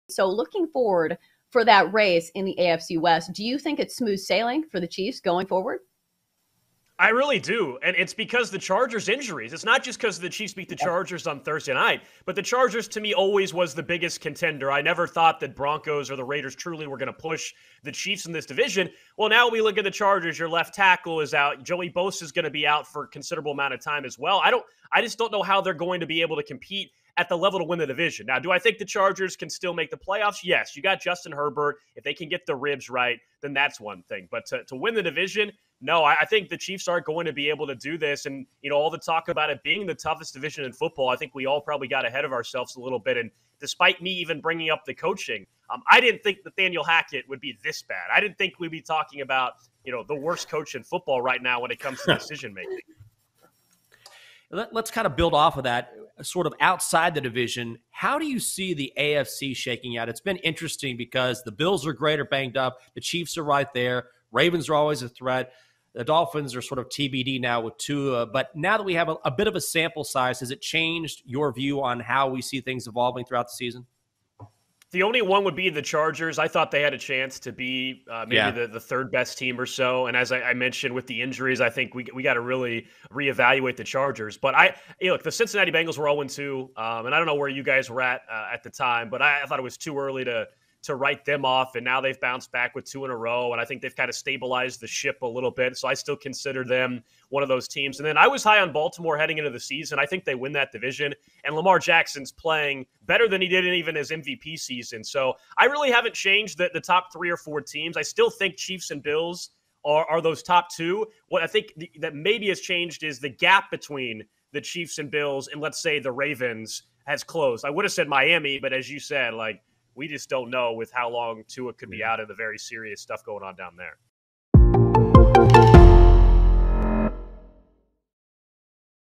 Why the time is now to buy in on Chiefs' futures
BetQL and Kansas City Radio 610's Alex Gold joins Michael Jenkins and Chelsa Messinger to break down the first few weeks of the Kansas City Chiefs' season and why the time is right to buy in on Chiefs' futures before they get too juicy.